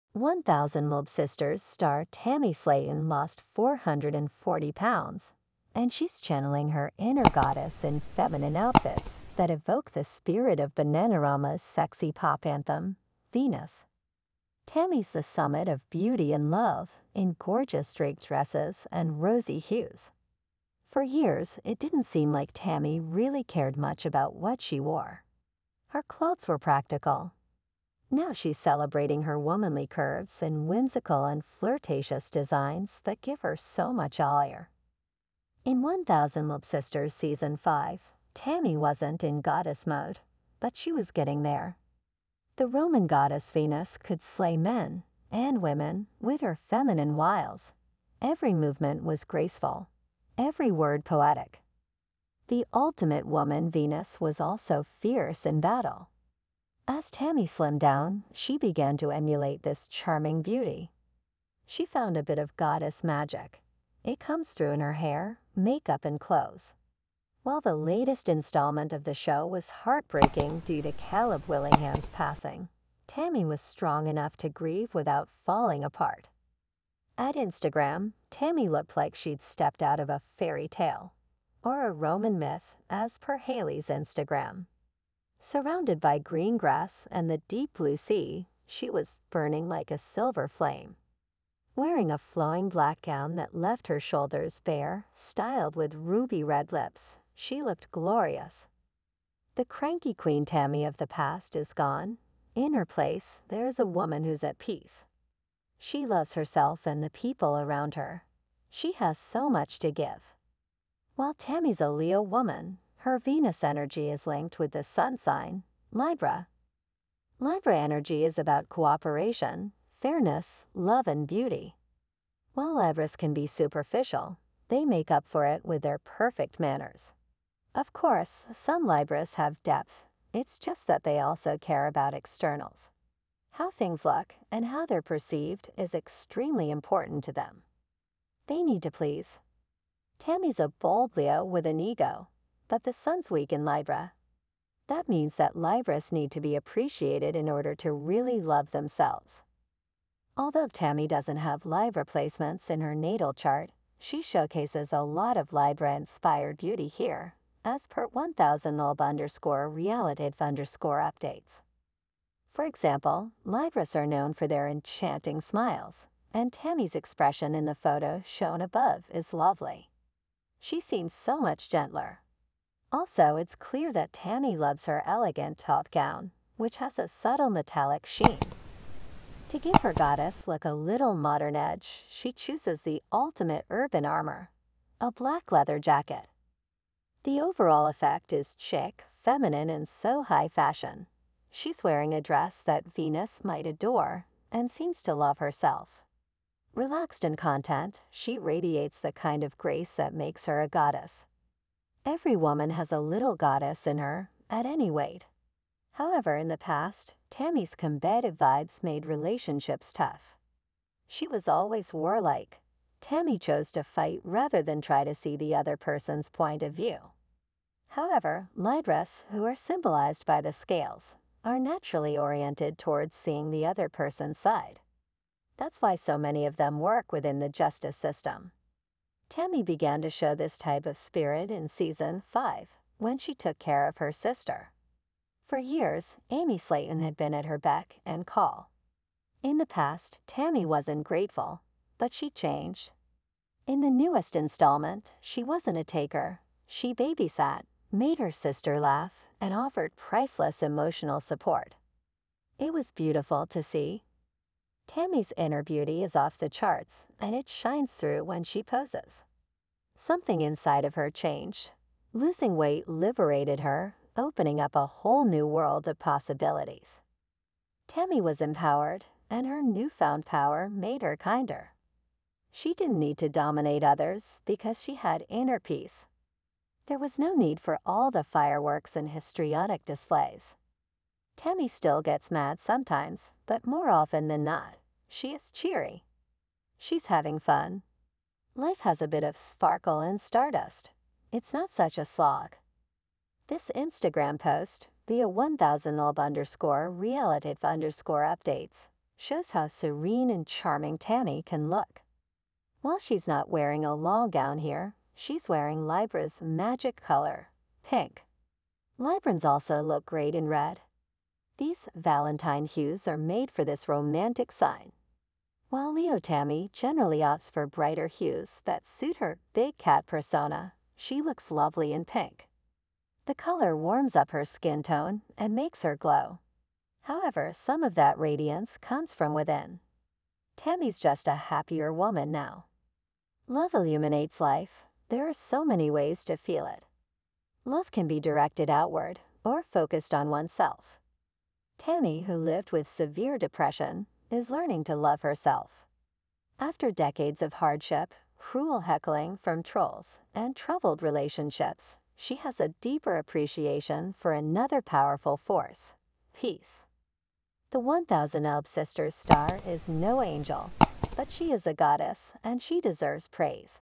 1000-Lb Sisters: "She's Got It!" Tammy's "Venus" In These Goddess Outfits After Extraordinary Weight
1000-Lb Sisters: "She's Got It!" Tammy's "Venus" In These Goddess Outfits After Extraordinary Weight Loss Milestone